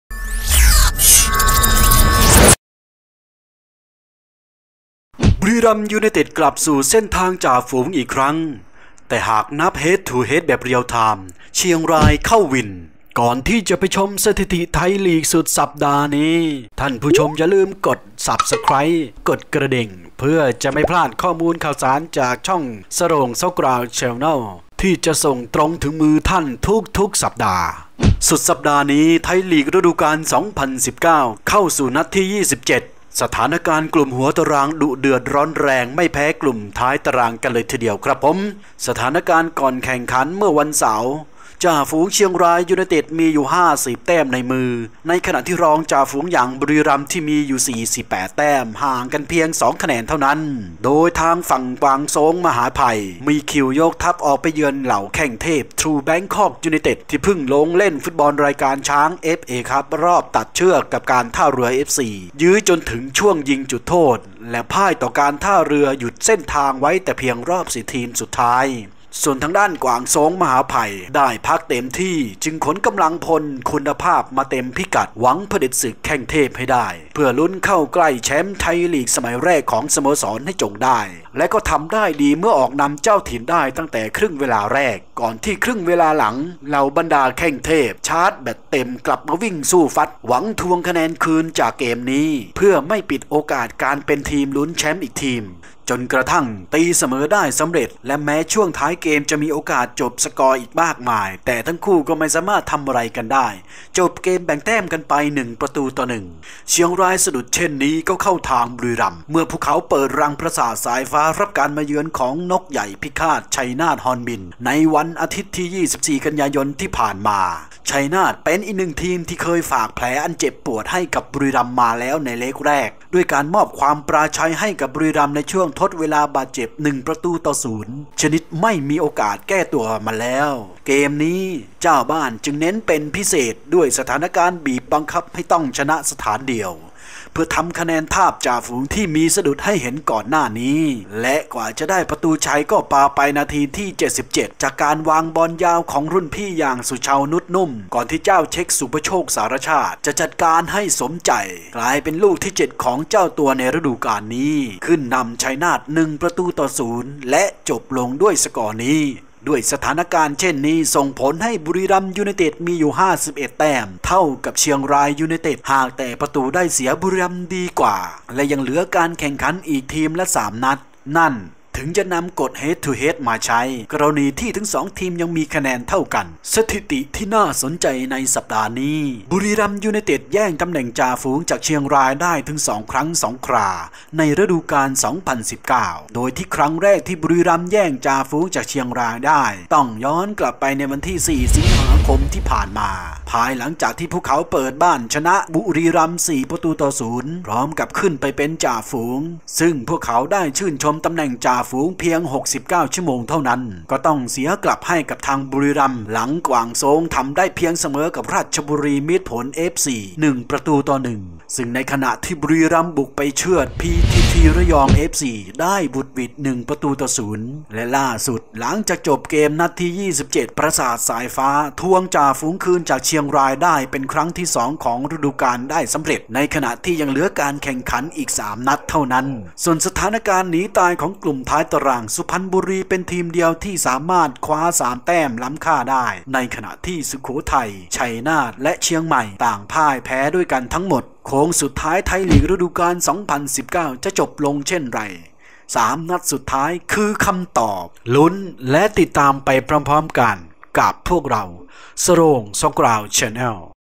ลุ้นไทยลีก 3 นัดสุดท้าย l คือประวัติศาสตร์หน้าใหม่ของฟุตบอลไทย
สุดสัปดาห์นี้ ไทยลีกฤดูกาล 2019 เข้าสู่นัดที่ 27 แล้ว สถานการณ์กลุ่มหัวตาราง ดุเดือดร้อนแรง ไม่แพ้กลุ่มท้ายตารางกันเลยทีเดียวครับ

เชียงรายสะดุดเช่นนี้ ก็เข้าทางบุรีรัมย์ เมื่อพวกเขาเปิดรังปราสาทสายฟ้า จัดการนกใหญ่พิฆาต ชัยนาท ฮอร์นบิล ขึ้นนำจ่าฝูง

สถิติสัปดาห์นี้ พบว่า บุรีรัมย์ ยูไนเต็ด สามารถกลับไปเป็นจ่าฝูงได้หลังจากแย่งกับเชียงรายมาแล้วถึงสองครั้งสองครา

ลุ้นไทยลีก 3 นัดสุดท้าย และไม่ว่าทีมใดจะเข้าวิน นั่นคือประวัติศาสตร์ฟุตบอลไทย